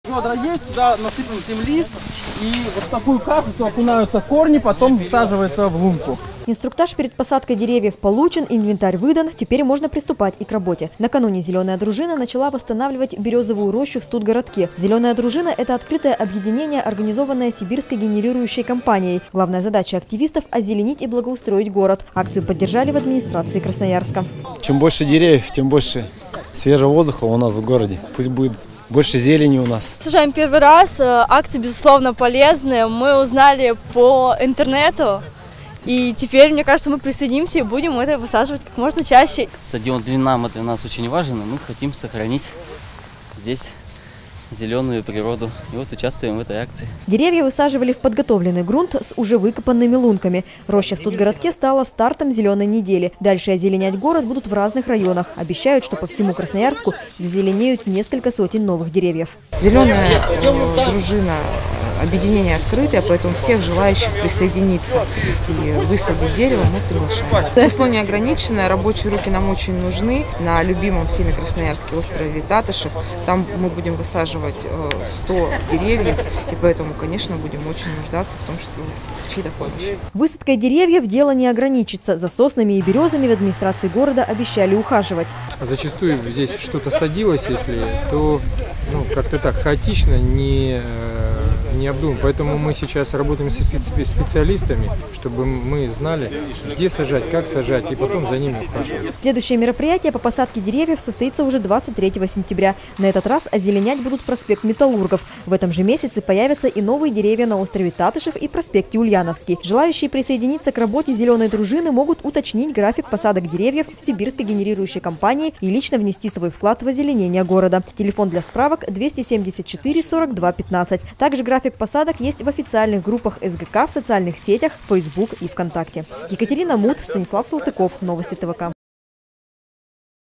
В Красноярске высадили 200 саженцев
В Березовой роще Студгородка появились новые, вязы, березы и сосны.